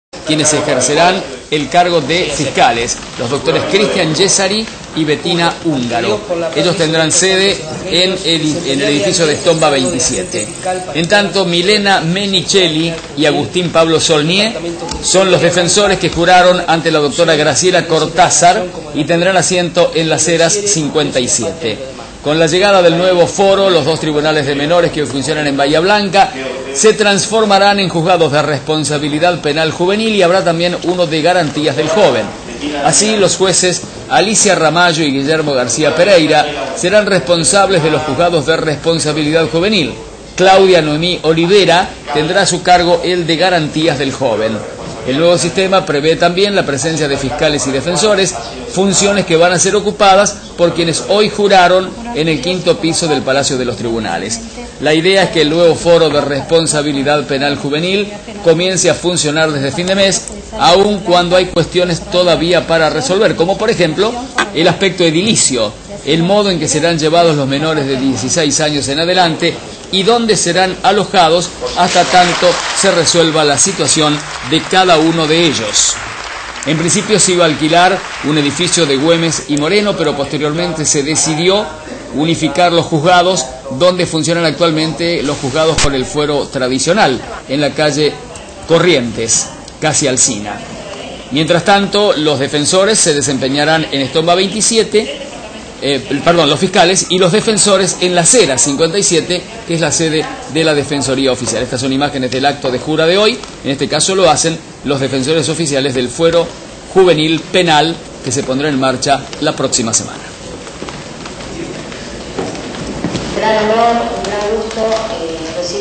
Juraron funcionarios del Fuero Penal Juvenil
Los nuevos Fiscales y Defensores tendrán sus oficinas en Estomba 127 y en Las Heras 57. Desde allí desarrollarán sus funciones en el marco del nuevo régimen legal para menores de edad.